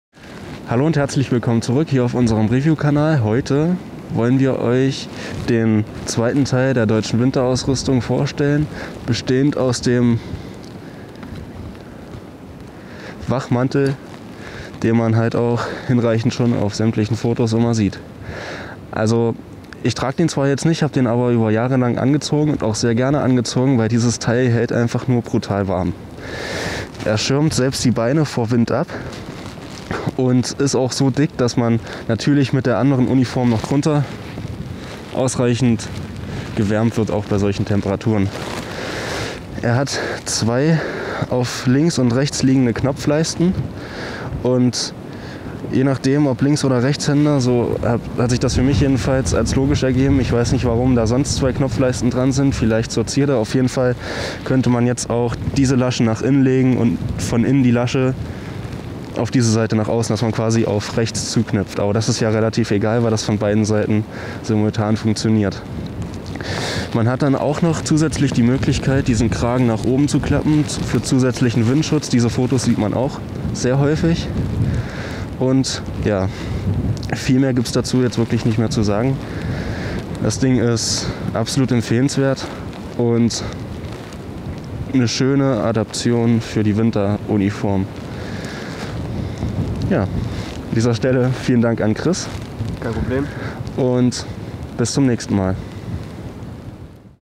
M40 Mantel für den Winter [Überblick]
Heute zeigen wir euch unseren M40 Mantel im Schneegestöber.